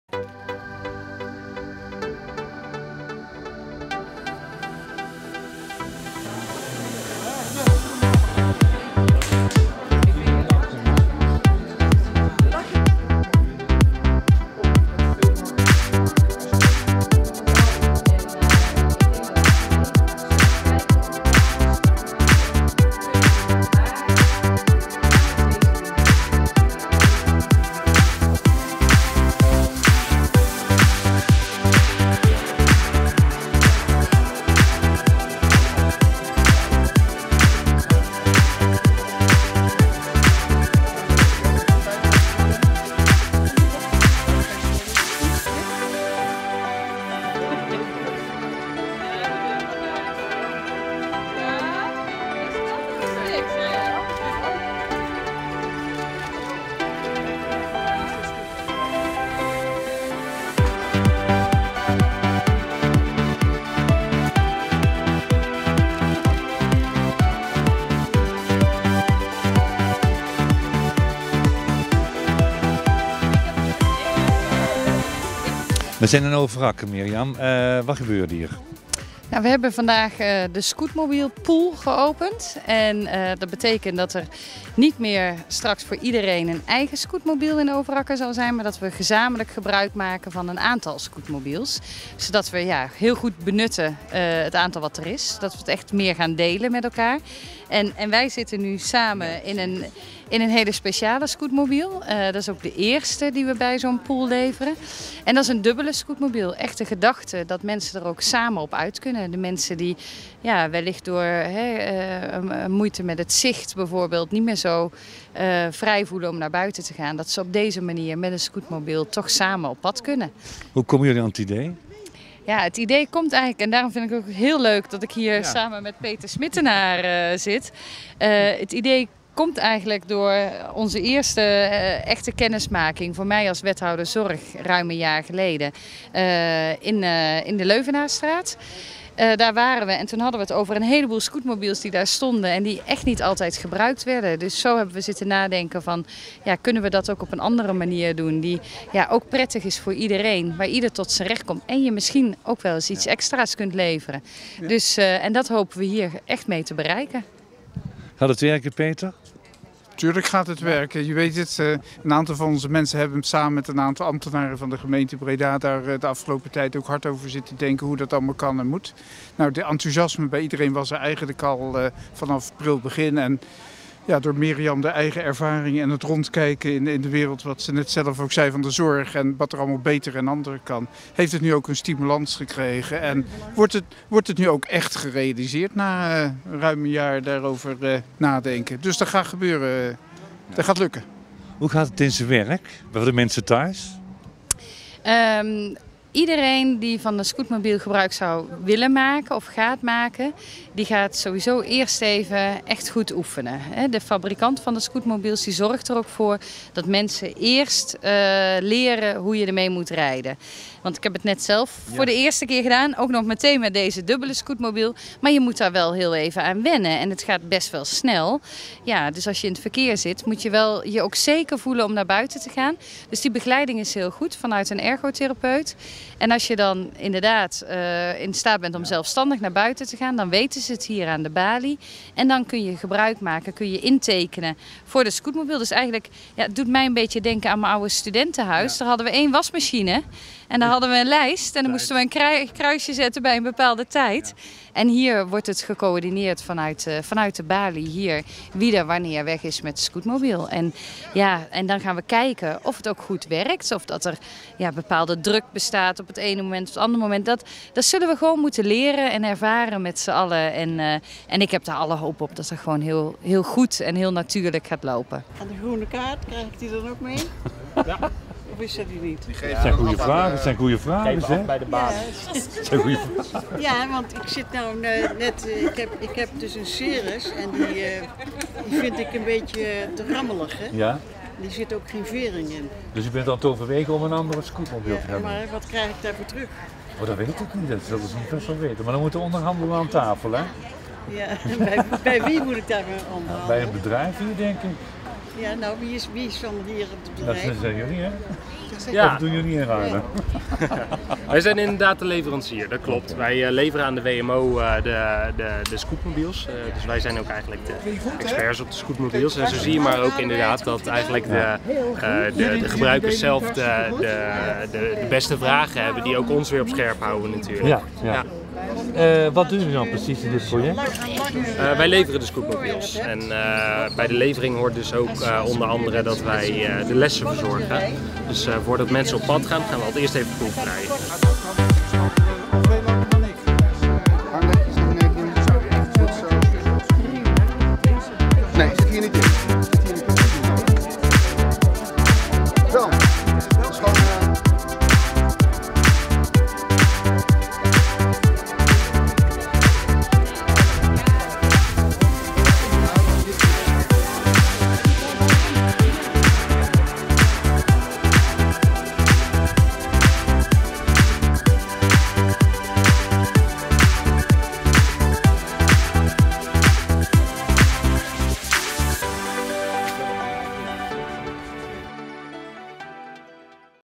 Presentatie Duo Scootmobiel
Voorlichting en presentatie van Duo Scootmobiel op Zorgcentrum Overakker met aanwezigheid van wethouder Meriam Haagh